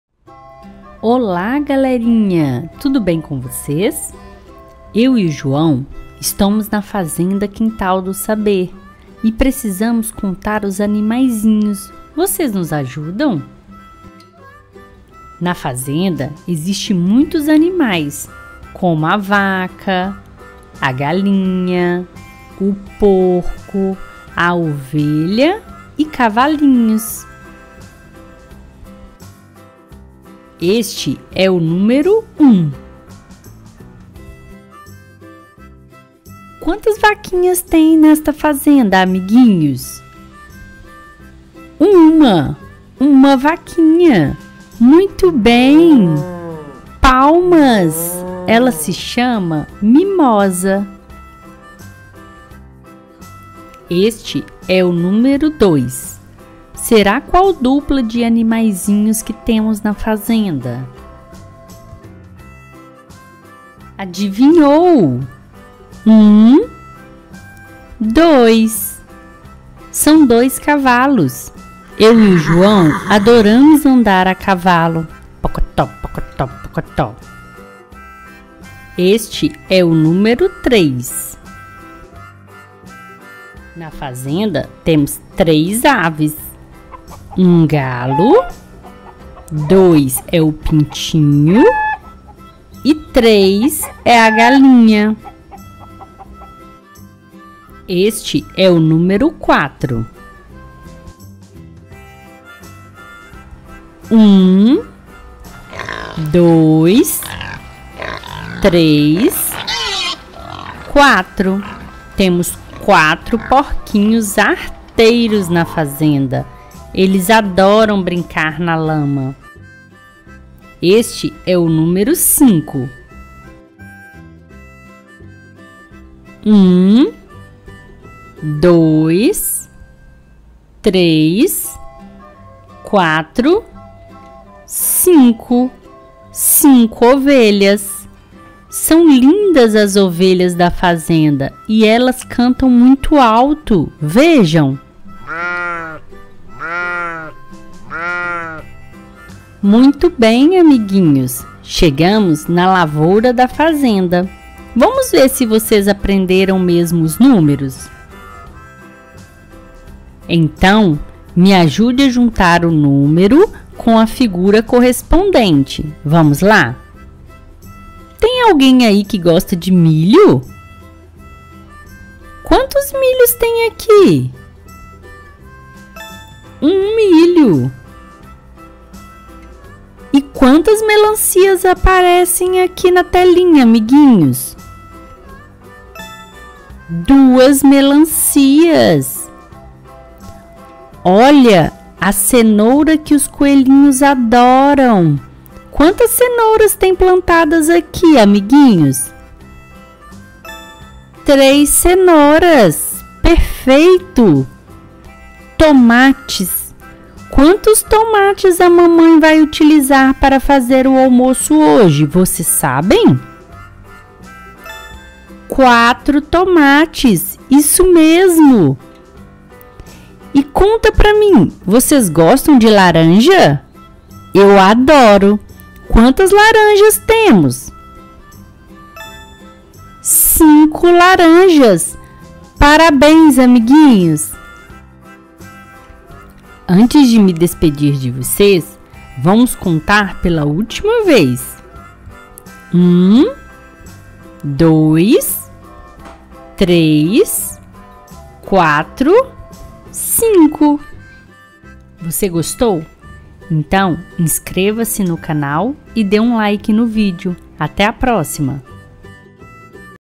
APRENDENDO A CONTAR DE 1 A 5 | ALFABETIZAÇÃO | EDUCAÇÃO INFANTIL
Vamos aprender a contar os números de 1 até 5?
Neste vídeo vamos aprender a falar e identificar os números de 1 a 5 de forma simples e divertida contando os animais.

- Este vídeo usa estes sons do freesound:

Vaca Muu por JuanLasso | Licença: Creative Commons 0
Horse Whinny, Close, A.wav por InspectorJ | Licença: Atribuição
Cacarejar de frango por Breviceps | Licença: Creative Commons 0

Porcos oinking por zachrau | Licença: Creative Commons 0

bee.aif por sofajoe | Licença: Atribuição
Batendo palmas por qubodup | Licença: Atribuição

Atribuições:
Desenhado por Freepik
Na data de publicação deste vídeo, conta premium.